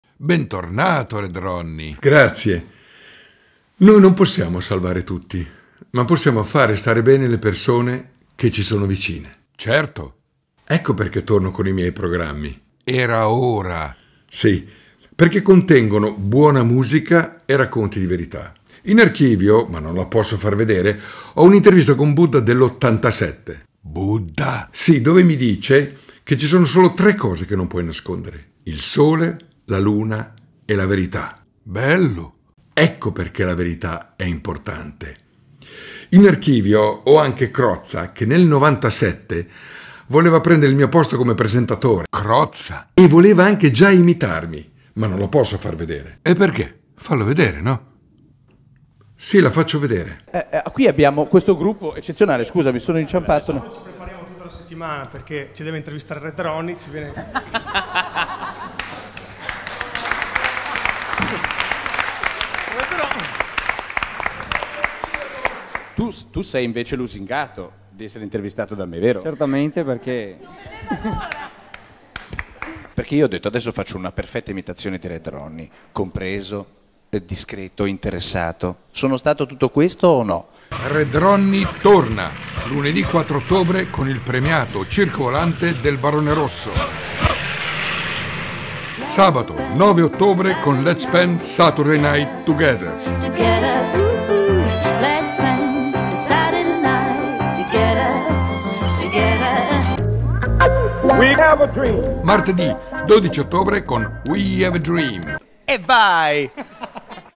Red Ronnie: ripartono dal 4 ottobre i miei programmi
Da lunedì 4 ottobre ripartirà il Premiato Circo Volante del Barone Rosso
da sabato 9 ottobre sarò nuovamente in diretta con Let’s Spend Saturday Night Together
da martedì 12 ottobre ripartiranno i collegamenti in WE HAVE A DREAM
Ho preso spunto dall'imitazione che mi fa Maurizio Crozza per realizzare lo spot. La citazione che faccio di Buddha è vera, come pure il fatto che in archivio abbia un Maurizio Crozza che le gennaio 1997 faceva le mie veci da presentatore e voleva già imitarmi.